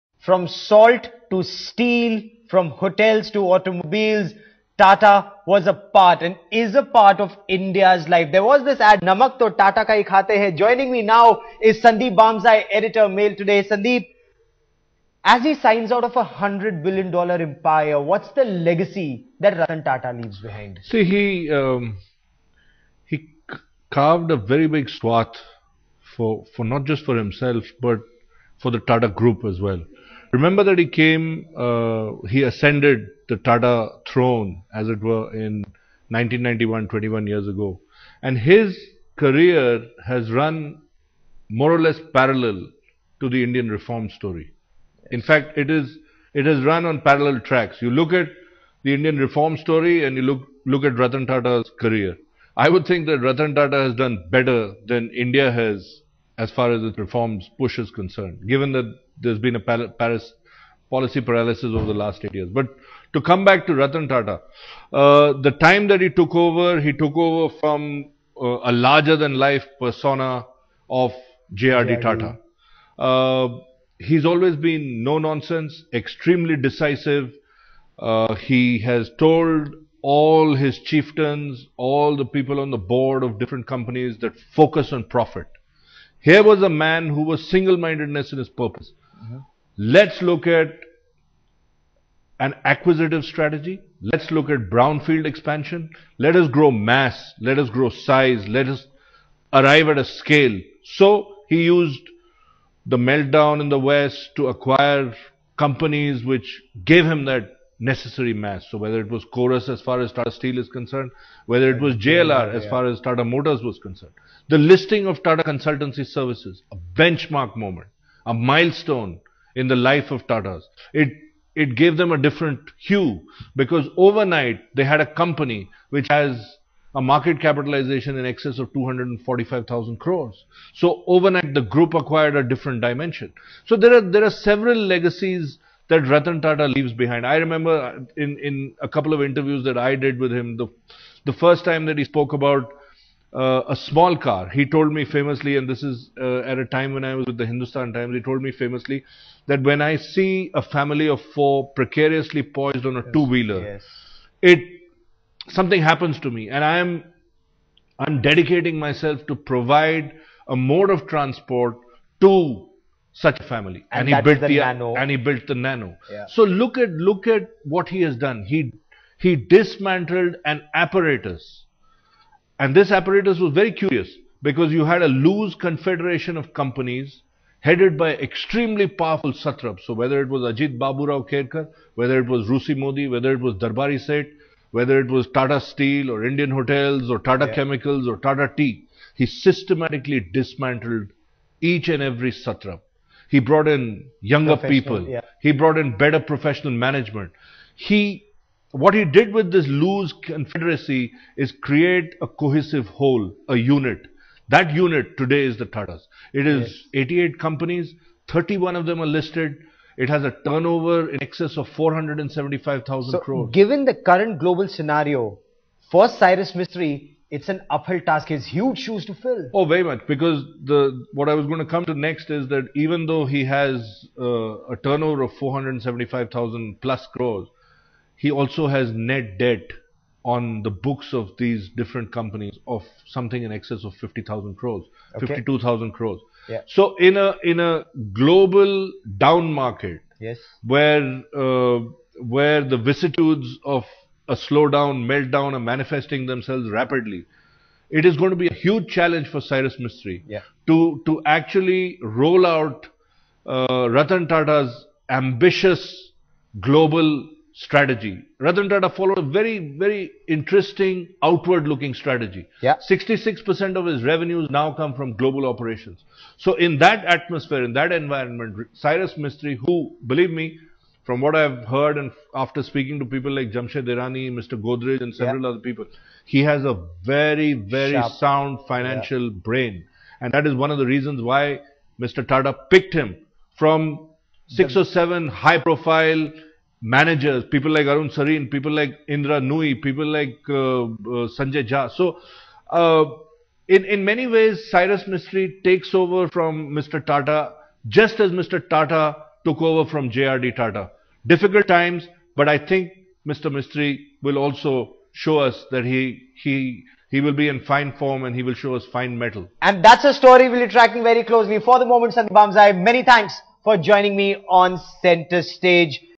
Ratan Tata retires, Cyrus Mistry to take over today
There was no event to mark the occasion, no speeches and the icon himself was not even there at Bombay House to see the transition, as Ratan Naval Tata retired as chairman of the $100-billion Tata Group, which he had joined 50 years ago.